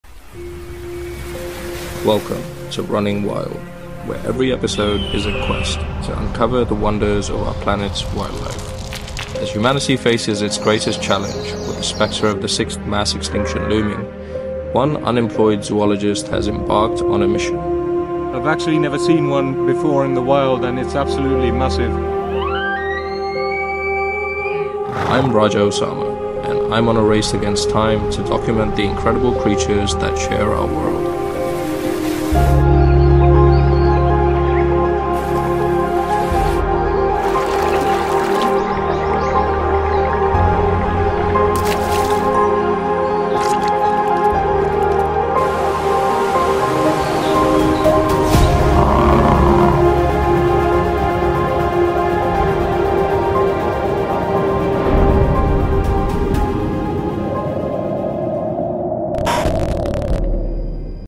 Journey To Document Wildlife Across The Globe | Running Wild
Join The Khanabadosh Boys on a journey filled with thrilling adventures as we travel across the world and explore the spectacular wildlife that calls this planet home. Tag along with Raja Usama, a zoologist on a mission to catalog as many species as possible before they perish in the current mass extinction event. This series is dedicated to shining a light on these magnificent species in their natural environments and the threats they face. This journey will take us from suburban backyards and cities, to some of the most remotest places on earth. From jungles, rivers, lakes, and mountains, to the seas and wild oceans. Become a part of the journey by joining our community of travellers

POSTING SCHEDULE:
- Videos : Monday - 8:00 PM